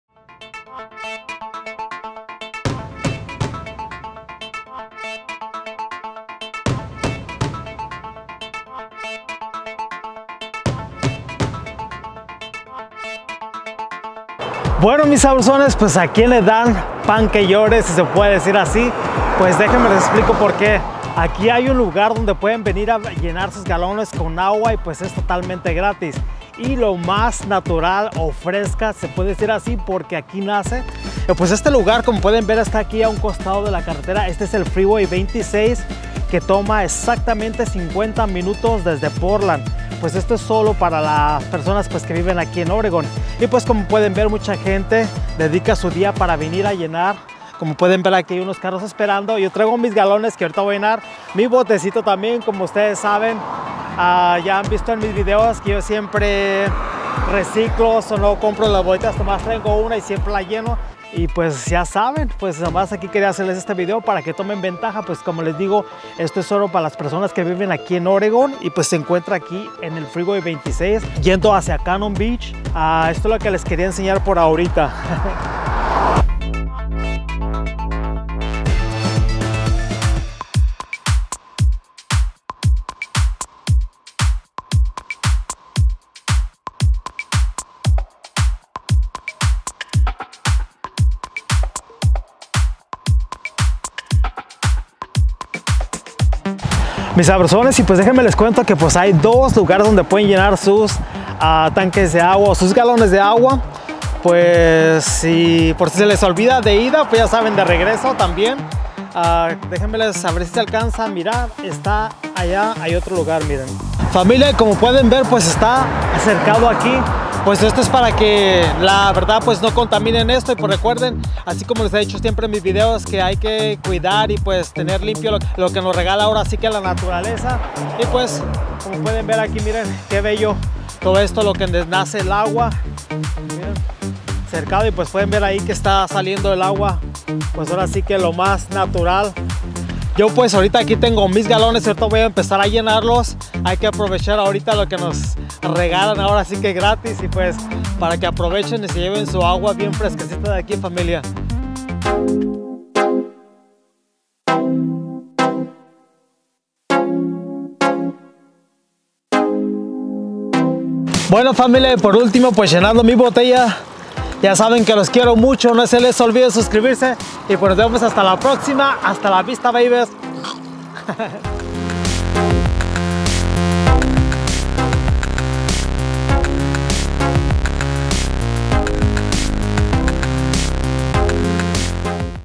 🔴 Explorando manantiales de agua fresca (Oregon)
Mis yolos para los residentes de Oregon ahorita que muchos estamos sin trabajo por la contingencia podríamos aprovechar esta agua gratis en el video les digo la ubicación nos es muy lejos de Portland y podemos aprovechar para despejarnos un poco tomando nuestras precauciones, espero les guste el video y nos vemos en la siguiente aventura.